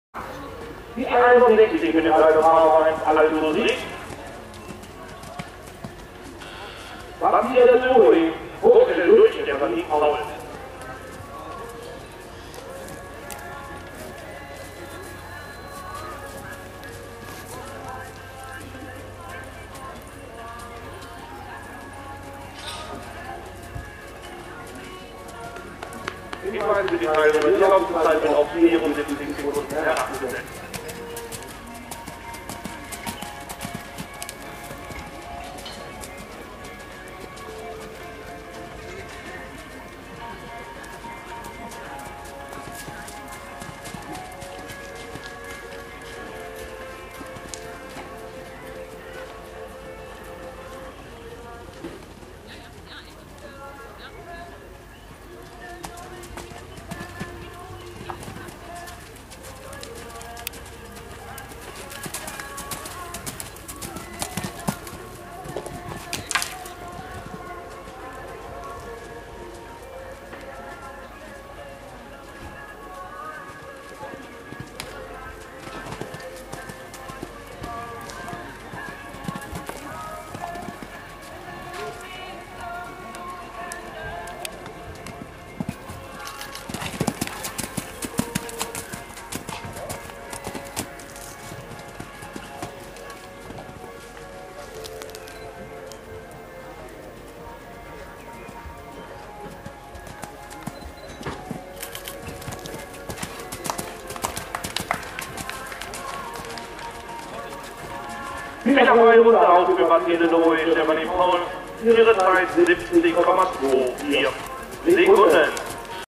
Batida du Rouet SPR M** Bad Schussenried 5-2012
Batida du Rouet geb. 2005, Oldenburger  Stute (Balou du Rouet x Arpeggio)
M** Springen in Bad Schussenried 5-2012. Reiter: Stefanie Paul,